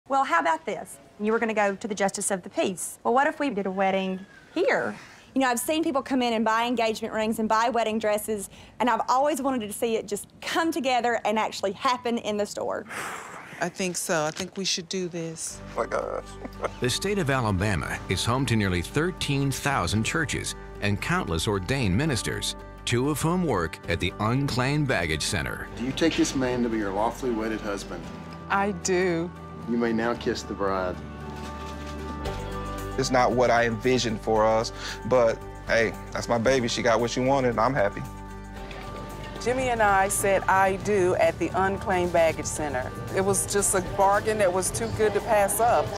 Woman Gets Married In An Unclaimed Baggage Center To Save Money!
After shopping in an unclaimed baggage centre for an engagement ring, she thinks it'll also be a very cheap place to have the wedding.